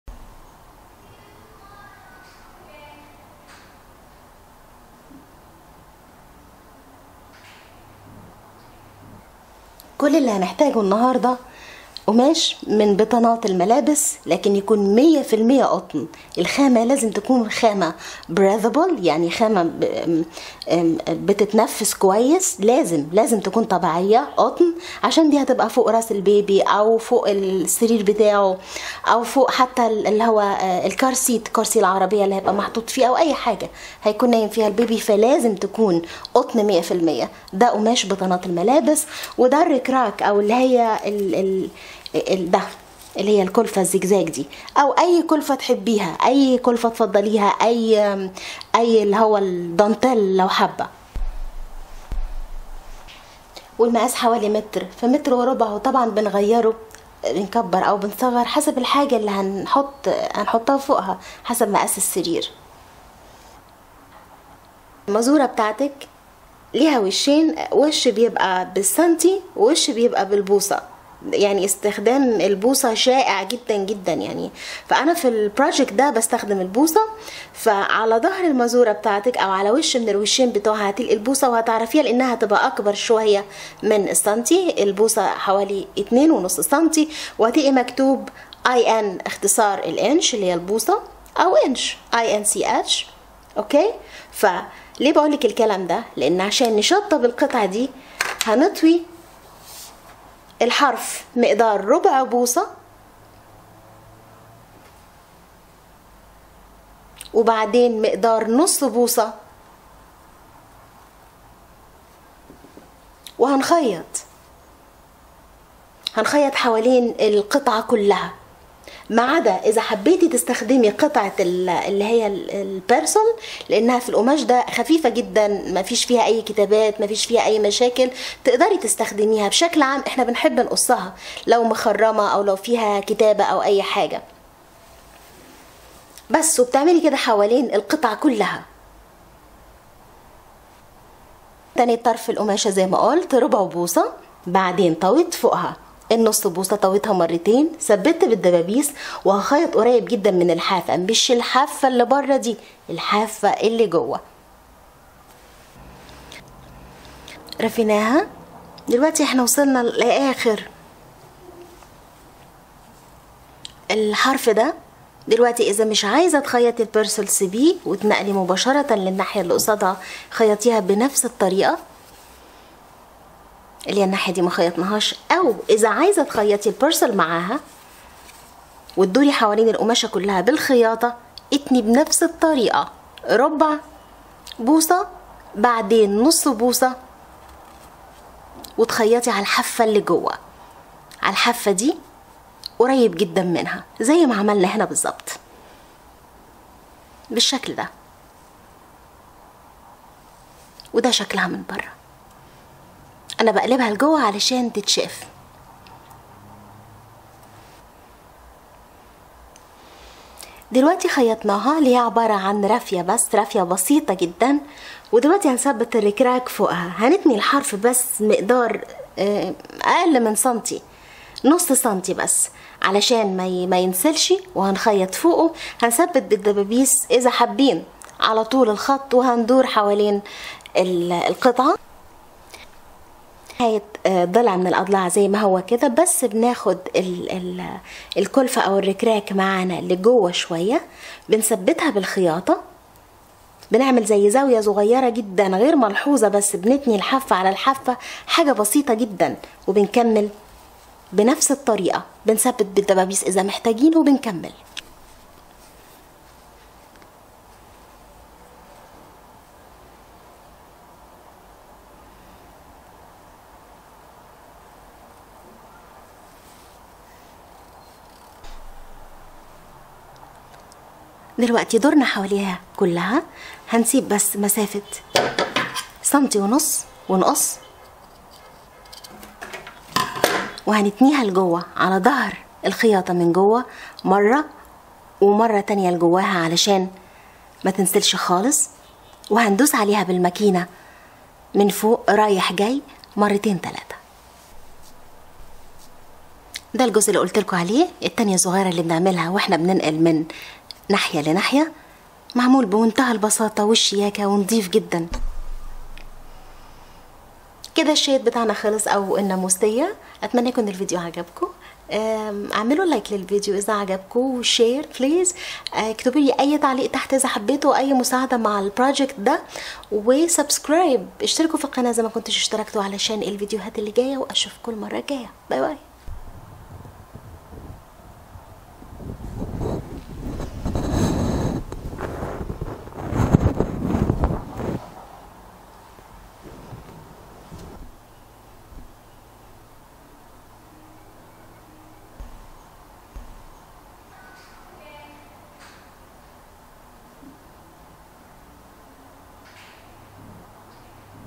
طريقة أسهل و أسرع ناموسية للبيبي.. لسريره أو عربيته أو للكار سيت
أهلا بالكل
عساكم بألف خير
 بشارككم طريقة عمل ناموسية للبيبي خليه ينام شوية ونرتاح
الأم المسكينة دي ترتاح شوية!!
مثالية لسريره و عربيته  والكار سيت خطوة بخطوة وسهلة جدا